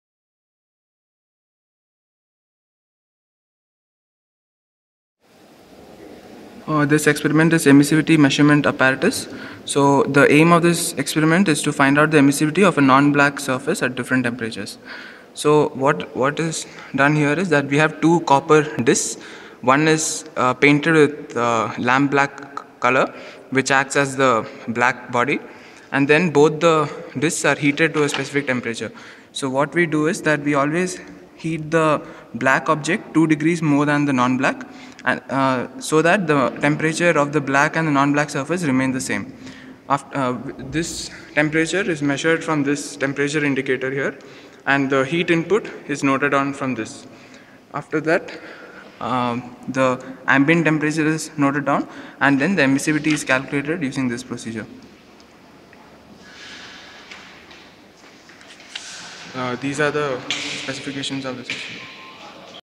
Emissivity of a Surface | Heat & Mass Transfer Lab | MechLabVideos
Emissivity of a Surface | Heat & Mass Transfer Lab
An experiment to determine the emissivity of a non-black surface.